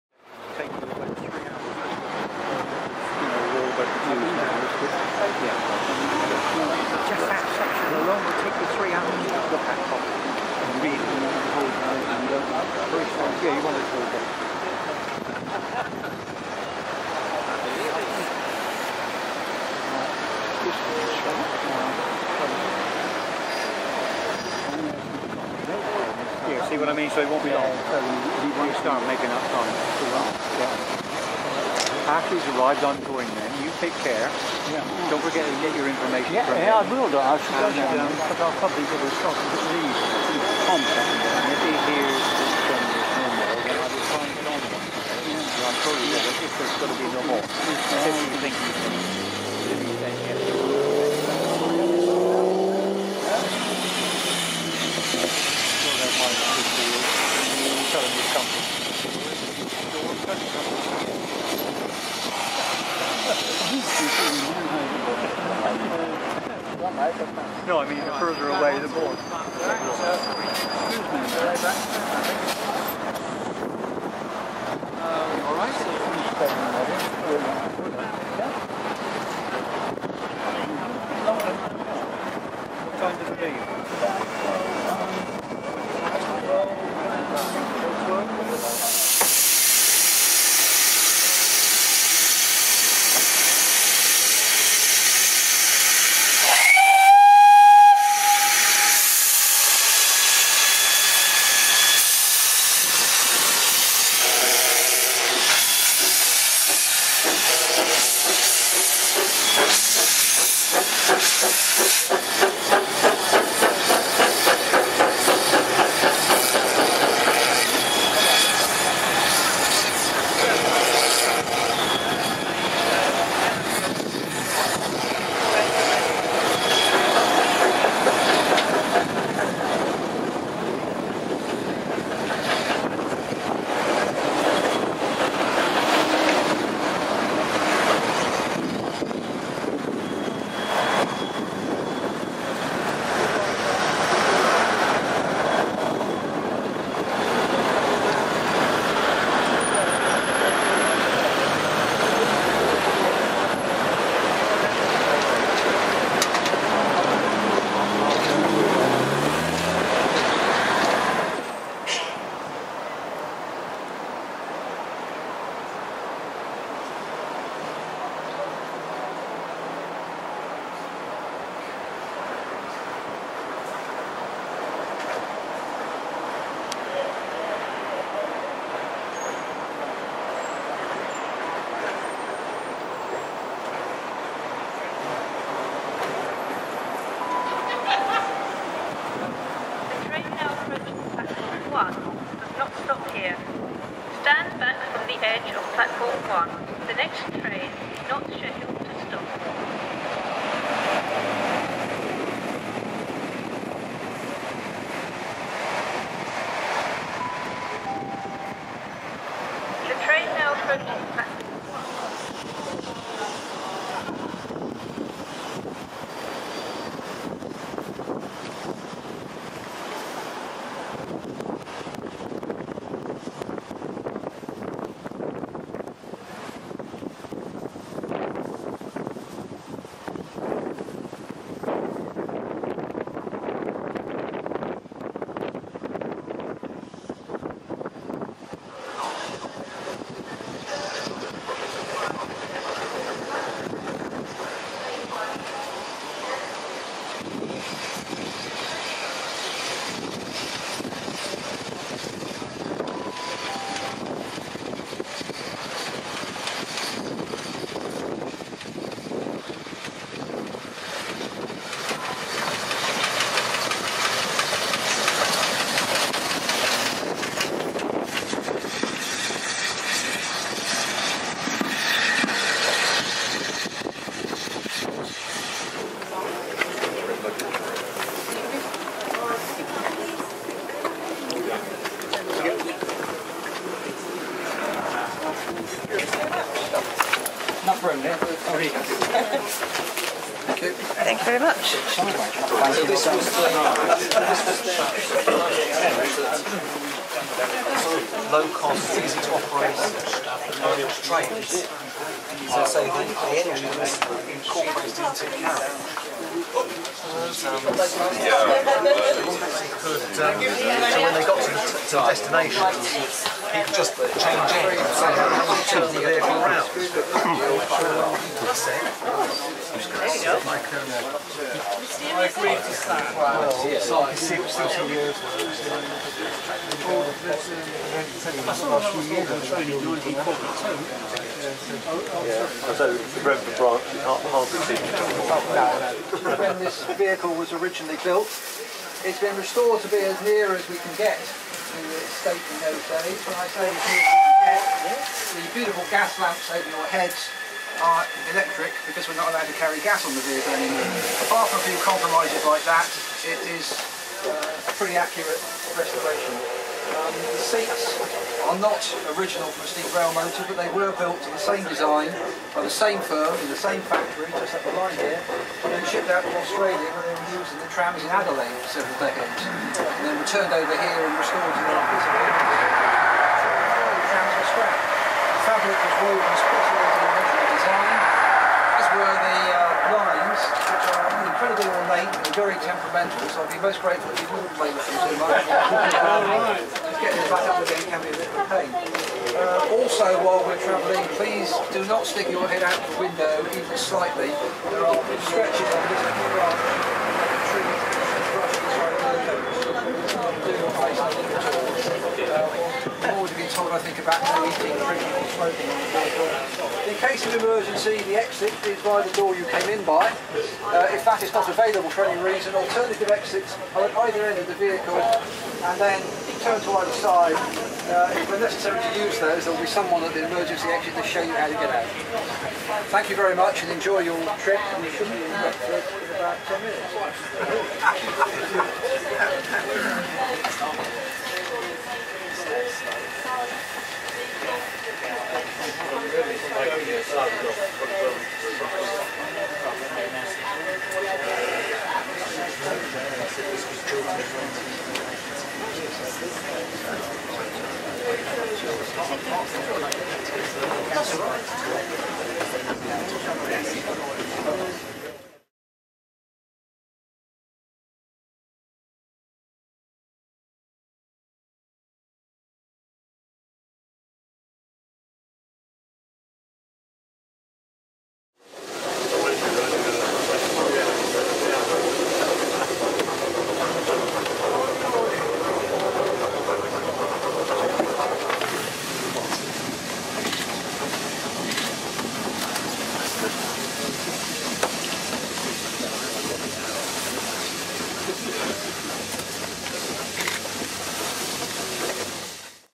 A ride on the GWR Steam Rail Motor
1908 GWR Steam Rail Motor Diagram R number 93 was running trips from Southall Platform no 1 down the Brentford goods line, by courtesy of The Great Western Society and First Great Western.   Note the footprints on the roof round the
 chimney, that had to be removed to get it under the bridges! Note Brunel's famous 'Three Bridges' where a road crosses the Grand Junction Canal with the railway in a cutting beneath the two. The Three Bridges bridge crossing is a unique transport intersection, that was designed and built by Isambard Kingdom Brunel. It was to be his last project before he died on 15 September 1859 just two months after its completion.